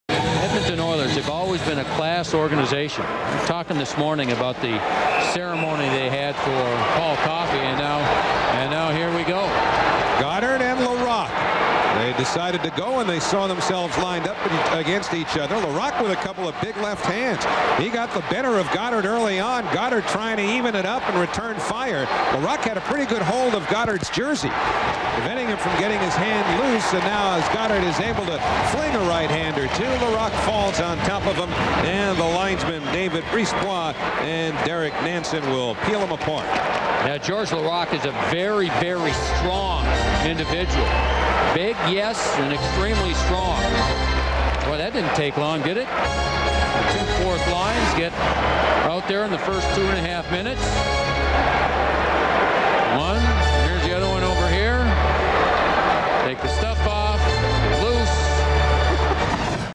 Georges Laraque vs Eric Godard Dec 10, 2005
Georges Laraque vs Eric Godard from the Edmonton Oilers at New York Islanders game on Dec 10, 2005.